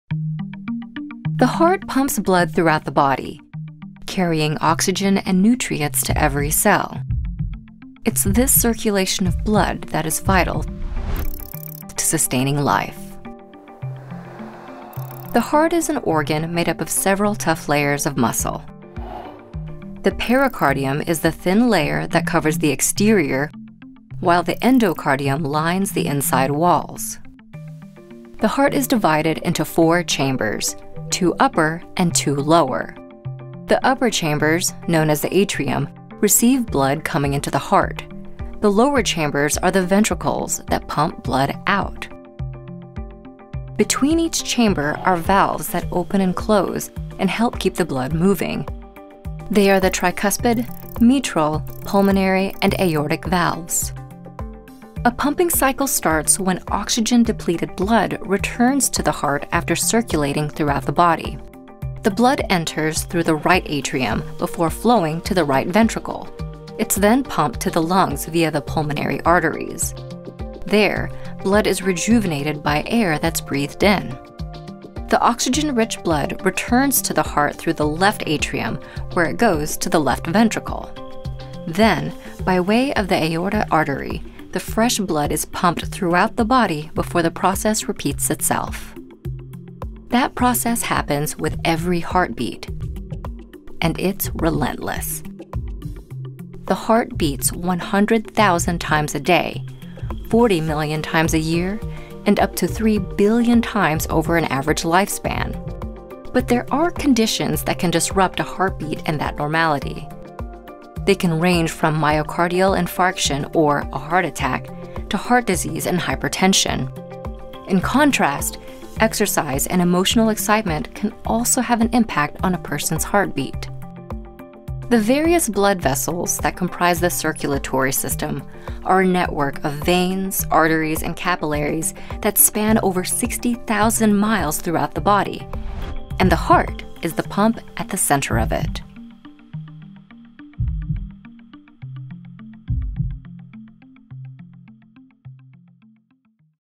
Alles wat je moet weten over het hart | National Geographic
Tijdens de gemiddelde levensduur van een mens slaat het hart ongeveer drie miljard keer. Kom meer te weten over de anatomie van het hart en hoe deze spier levensgevende zuurstof door het lichaam pompt.

Meer National Geographic?